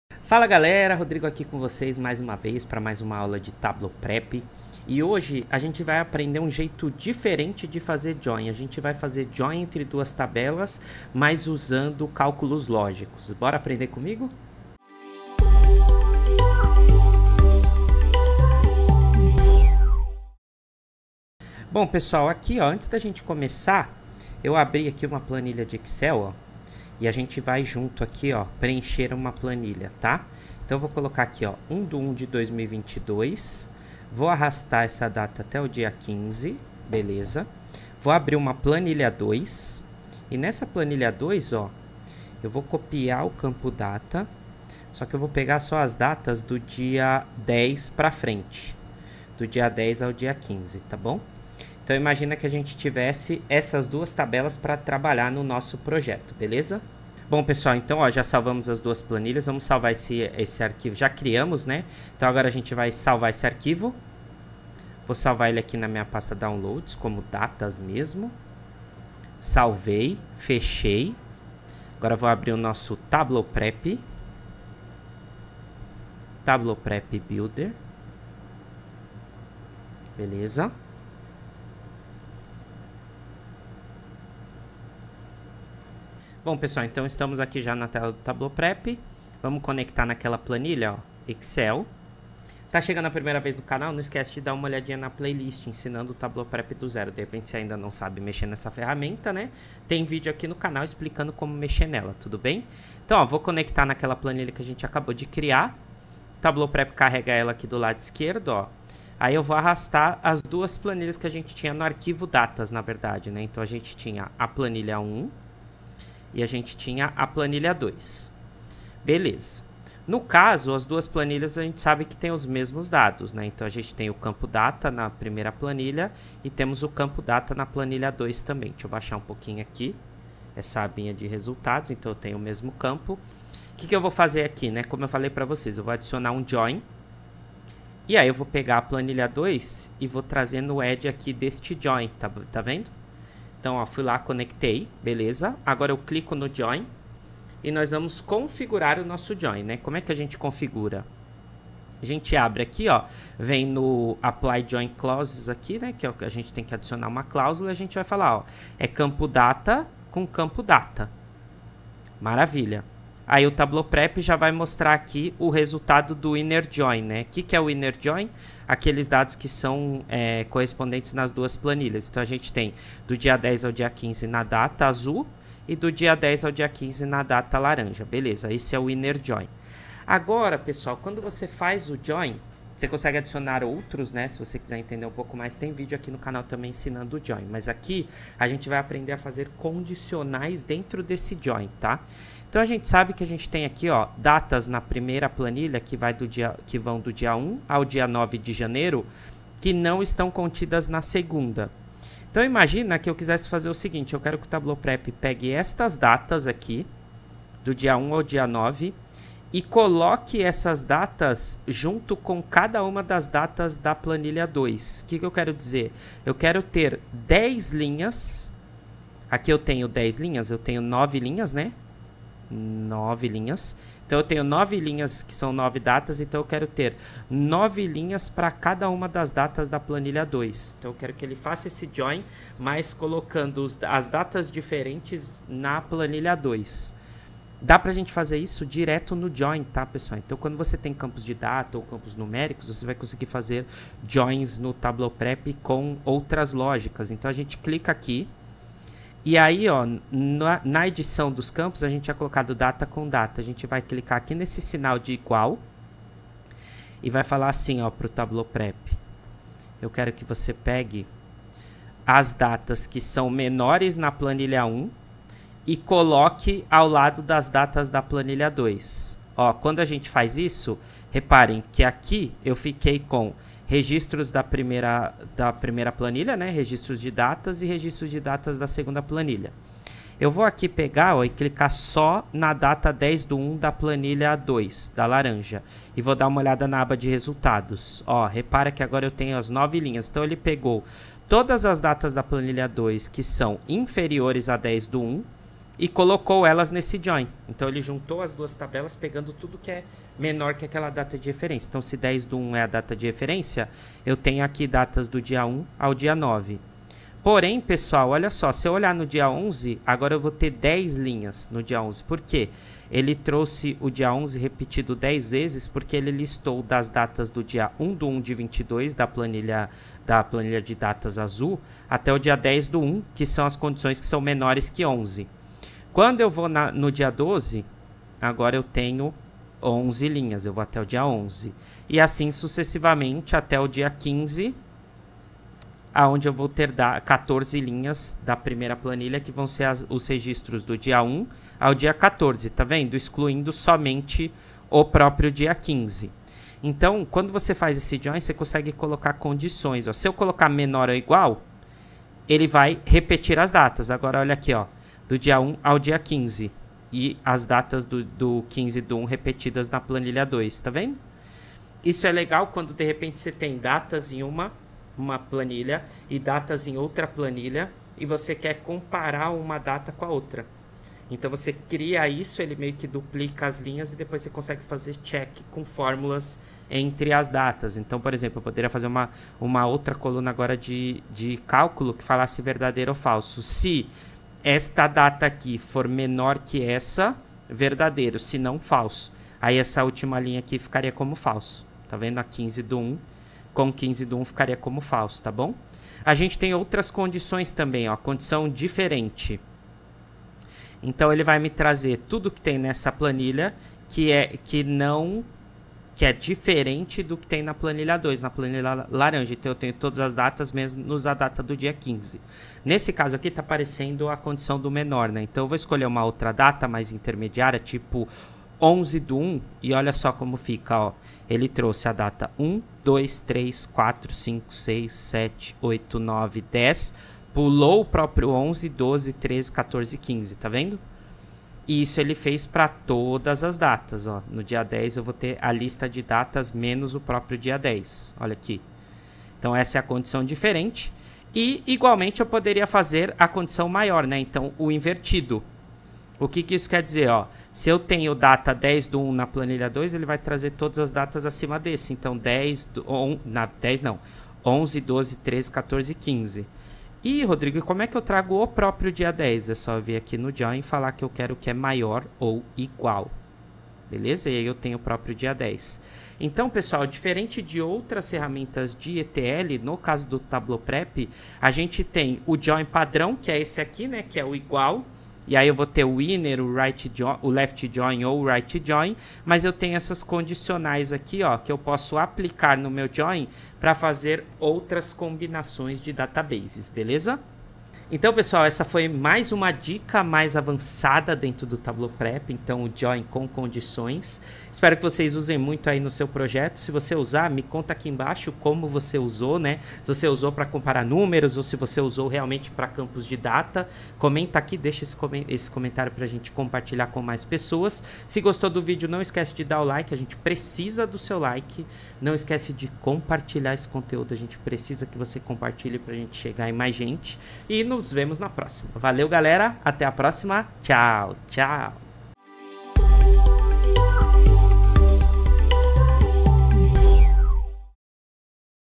TABLEAU PREP - Essa você não esperava! JOIN com CONDICIONAIS
Nesta segunda, o Rodrigo ensina como usar condicionais no Join.
Assista e aprenda com a gente!

🔷 Interesse em conhecer mais? ACESSE AGORA!

00:00:00 Proposta do Vídeo
00:00:21 Criando Base de Dados
00:01:31 Configurando o Join
00:02:59 Inserindo Condicionais
00:08:07 Conclusão